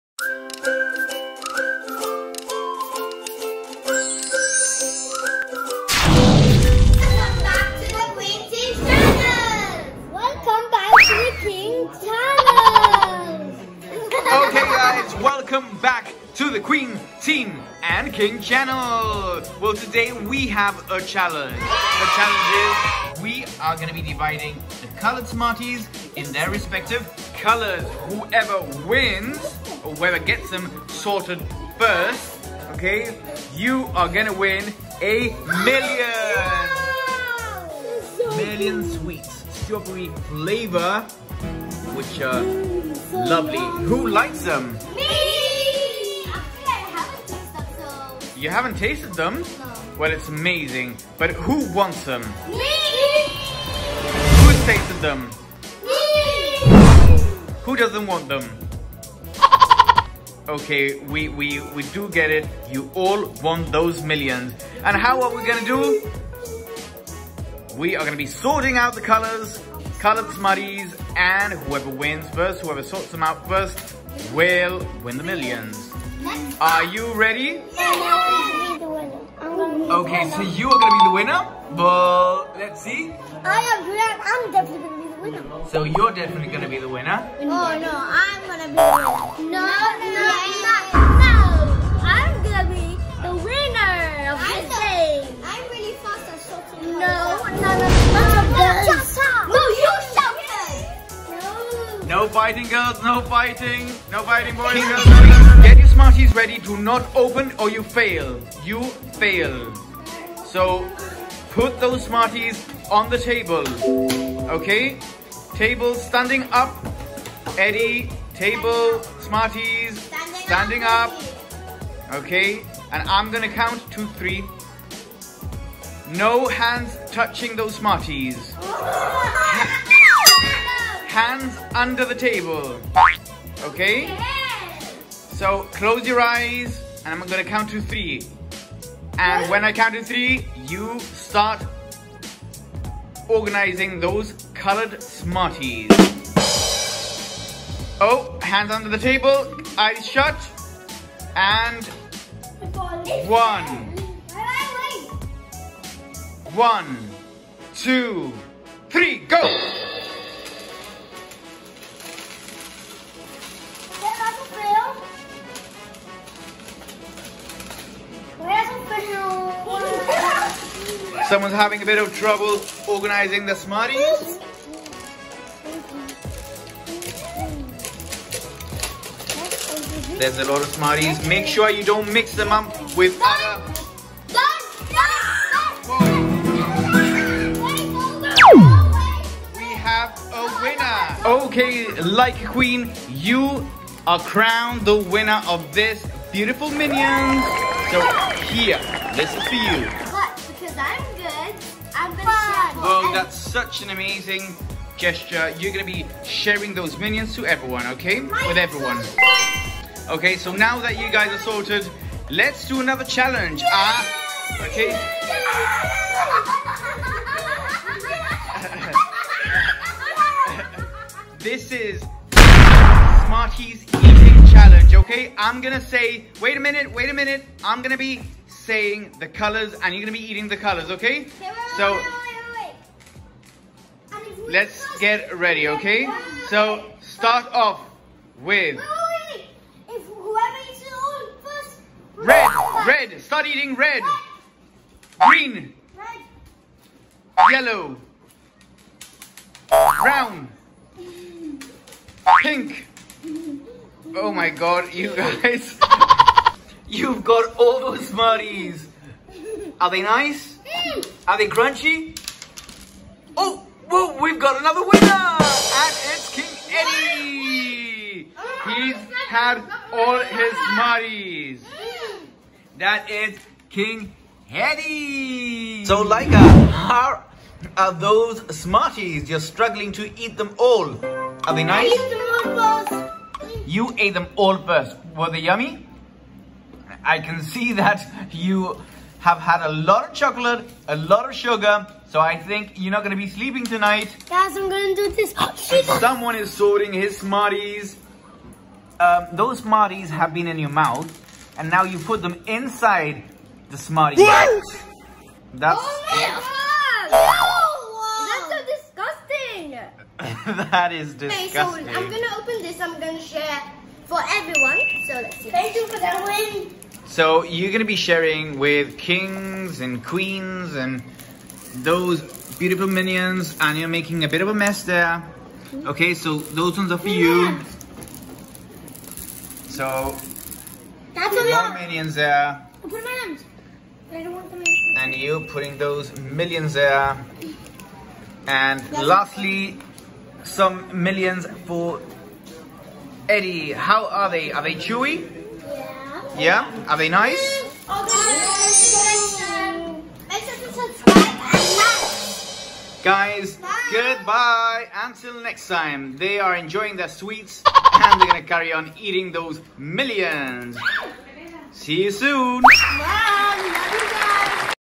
Candy Challenge
King Teens Vs Queen Teens..fighting for their candy! Don’t miss this new episode, they eat so much candy to win more candy!